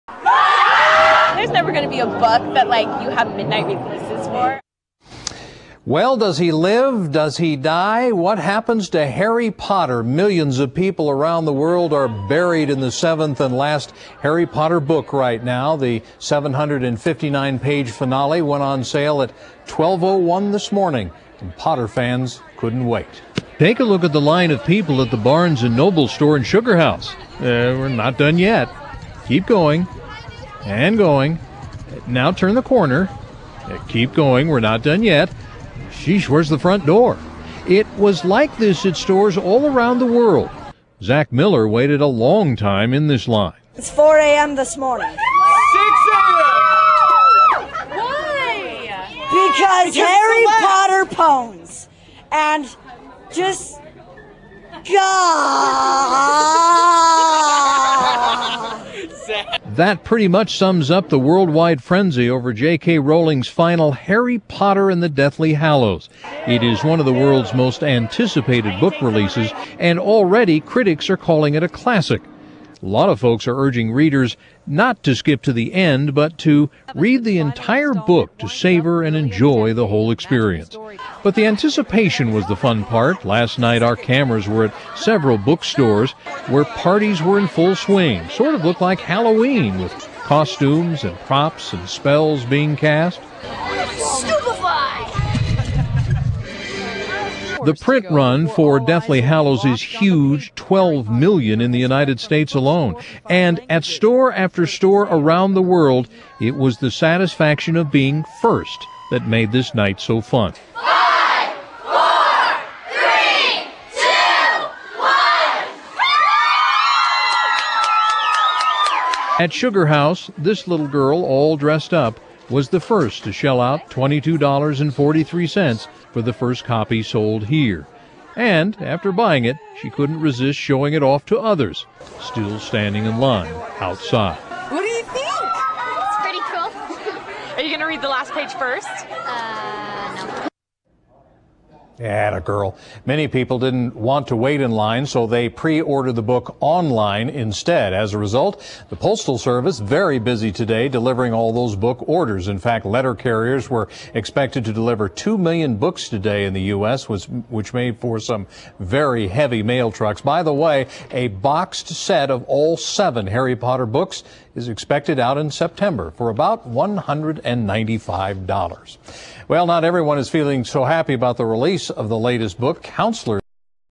The Wait is Over for Harry Potter Fans－哈迷们的等候结束了
The Wait is Over for Harry Potter Fans－哈迷们的等候结束了－July 21st, 2007 KSL News | The transcript is 欢迎登陆 查看解说词文稿。